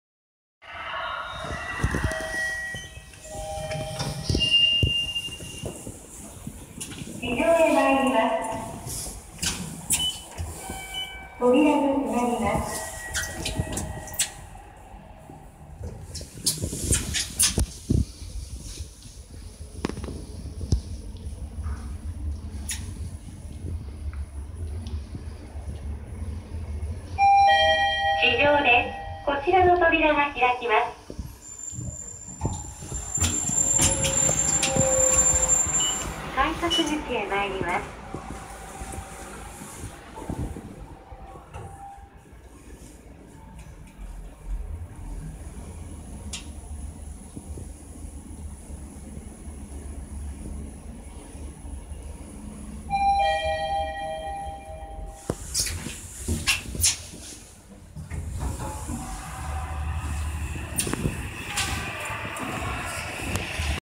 港南中央駅のエレベーター（地上側）
港南中央駅のエレベーターです。
こちらは地上側で駅舎仕様となっています。セーフティシューは黄色です。

改札内側：未撮影

データ
メーカー：三菱
構造：ロープ式
モデル：三菱駅舎用エレベーター
用途：乗用
定員：11名
積載：750Kg
設置数：1
停止階：改札口・地上
車椅子操作盤：あり
アナウンス：あり
チャイム・ベル：あり
ボタン確認音：あり